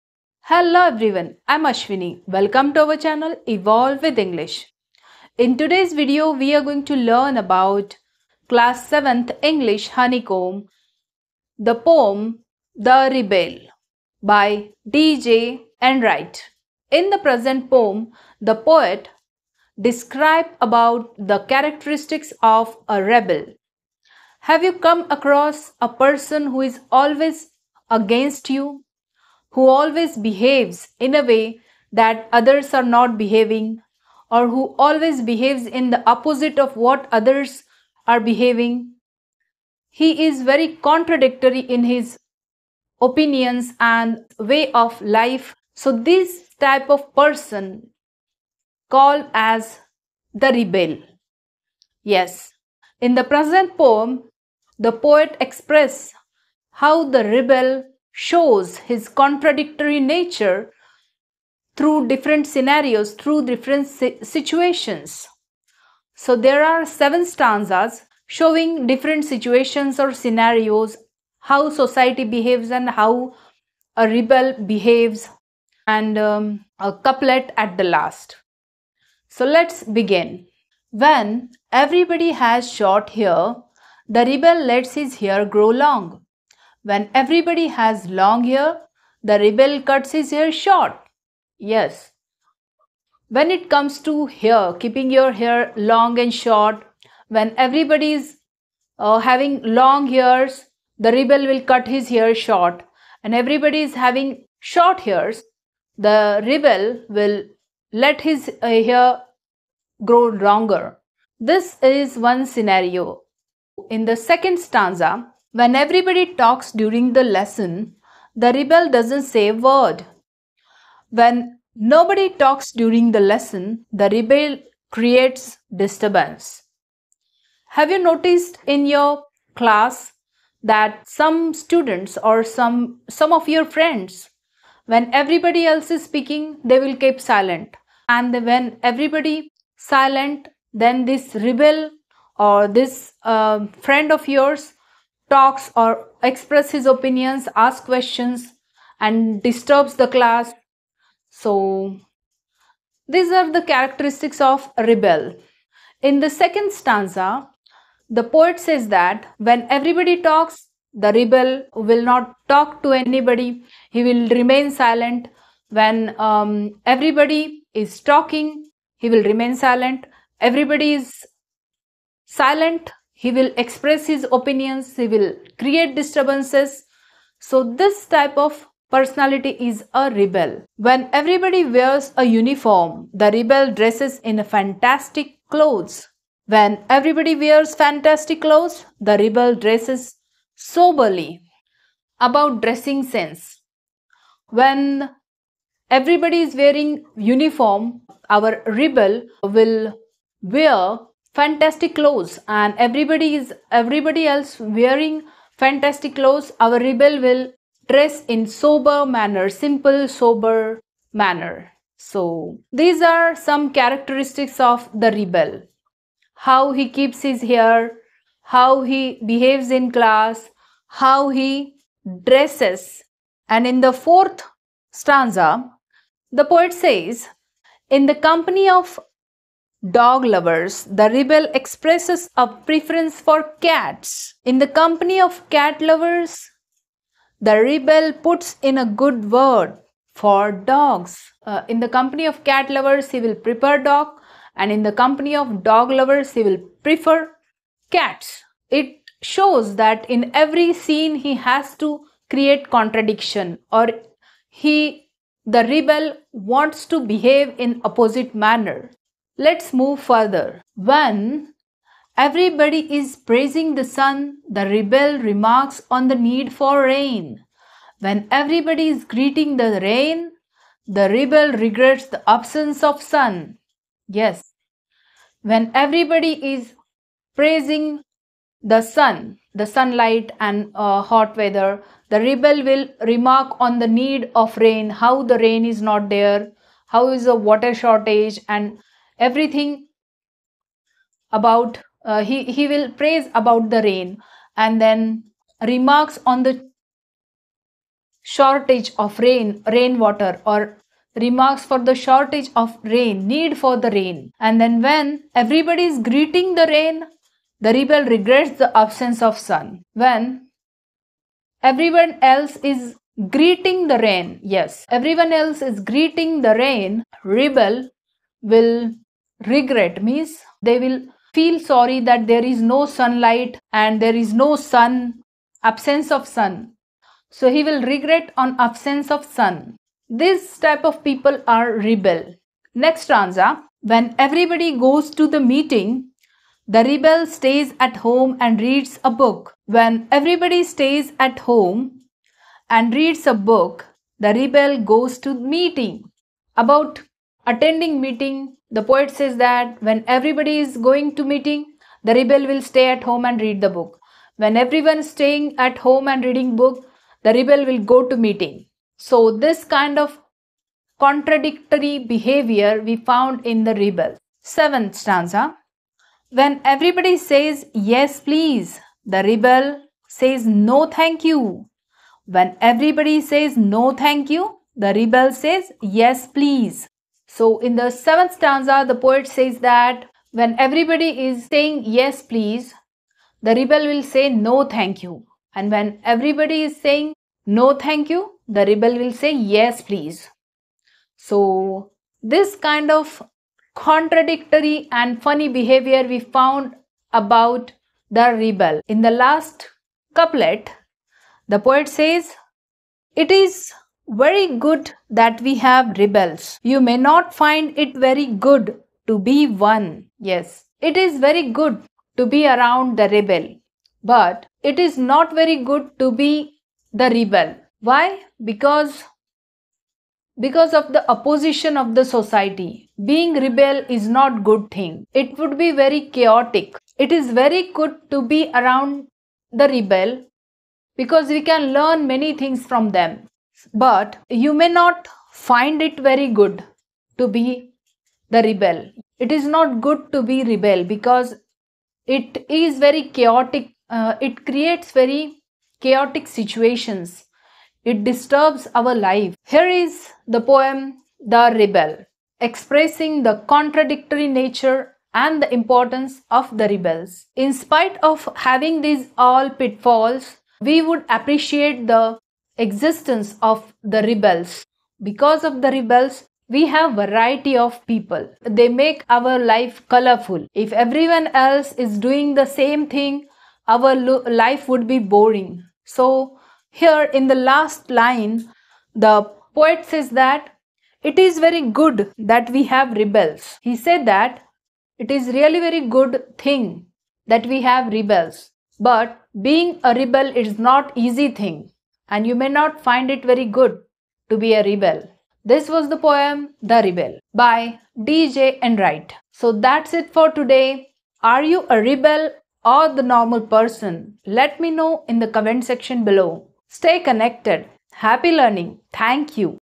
The Rebel...class 7 ,English, Honeycomb.
Hello Friends 🤗
In this video, we are learning about the characteristics of a rebel through the poem 'The Rebel ' from class 7 English Honeycomb.